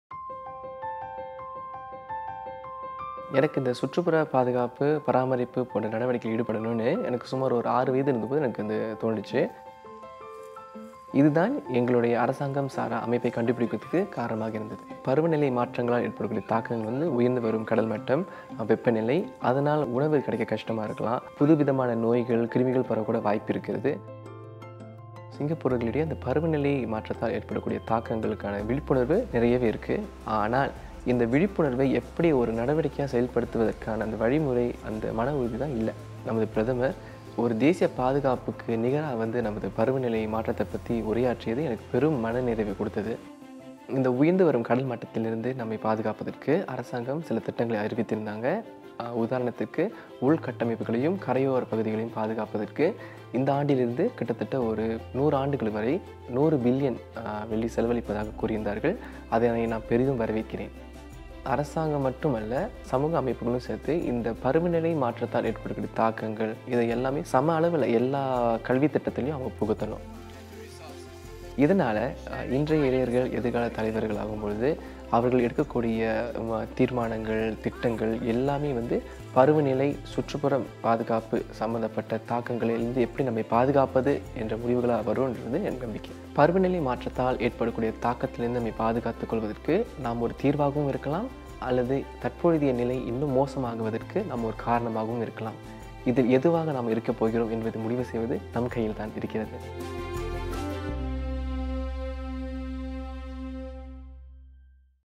NDR2019| Why climate change matters to me [Tamil]
உயர்ந்து வரும் கடல் மட்டதால் சிங்கப்பூர் எவ்வாறு பாதிக்கப்படும்? தேசிய தின கூட்ட உரையில், பிரதமர் லீ சியன் லூங் பருவநிலை மாற்றத்தை பற்றி பேசியது, சுற்றுசூழல் ஆலோசகராக பணிபுரியும் இராமநாதன் திரைராஜு போன்ற இளைஞர்களுக்கு மனநிறைவை கொடுத்தது.

மேலும் பருவநிலை மாற்றம் குறித்த NDR2019 அறிவிப்புகளுக்கு, செல்லுங்கள்.